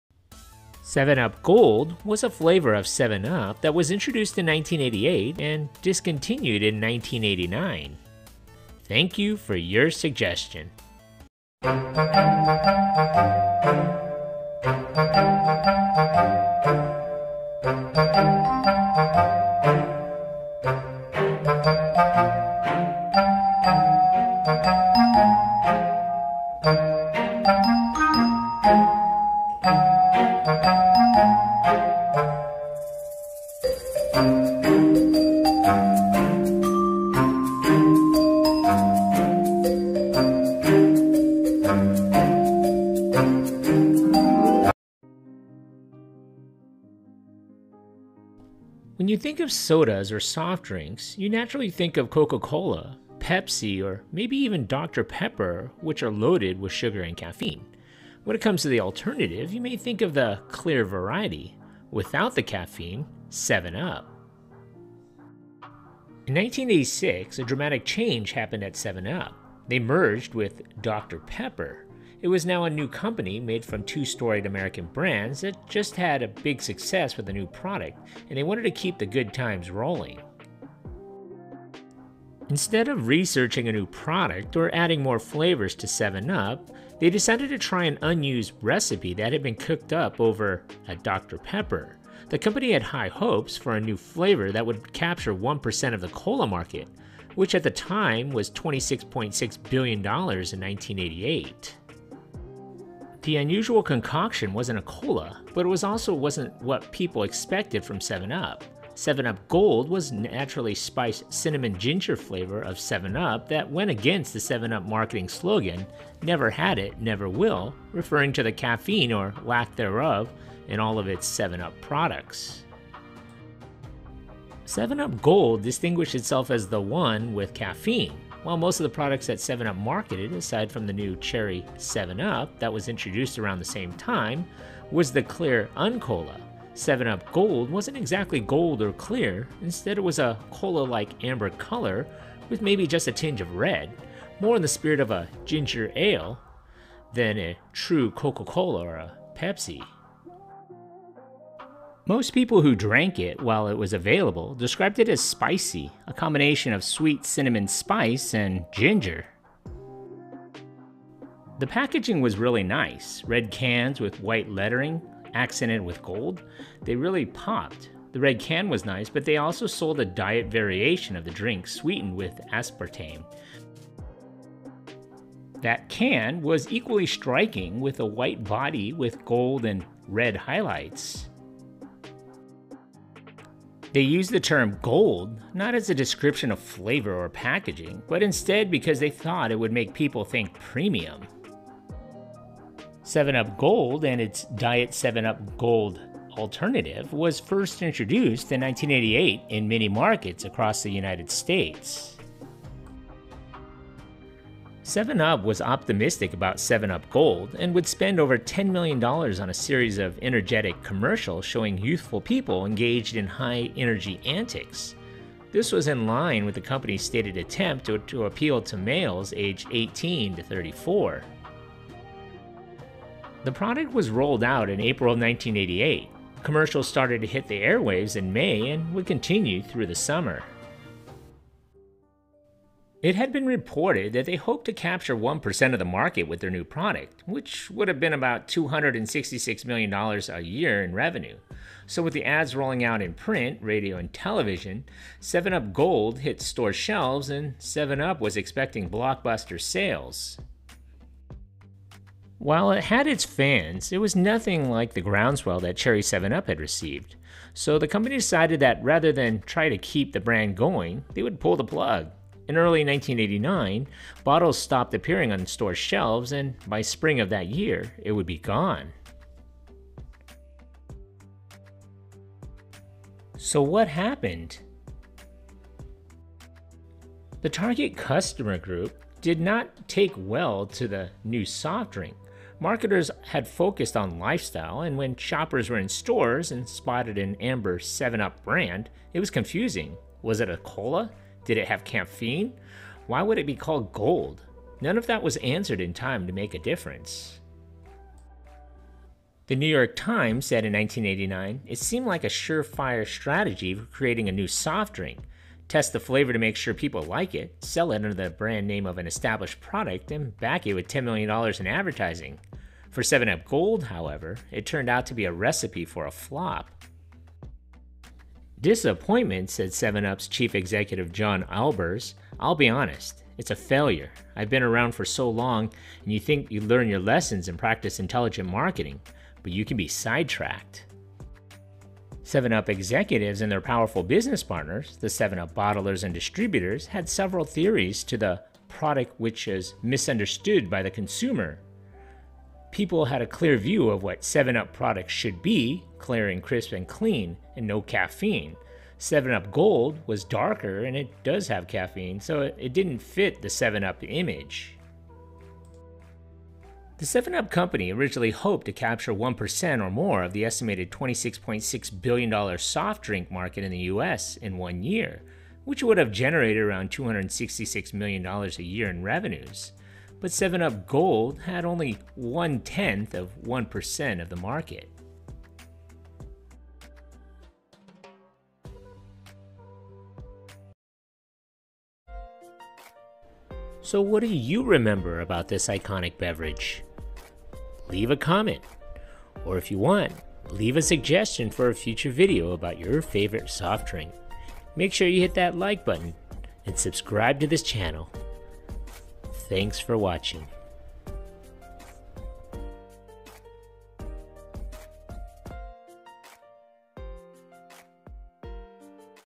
Do You Remember 7 Up Gold? A Soda History.
7 Up Gold was a naturally spiced Cinnamon Ginger flavor of 7 Up that was introduced in 1988 and discontinued in 1989.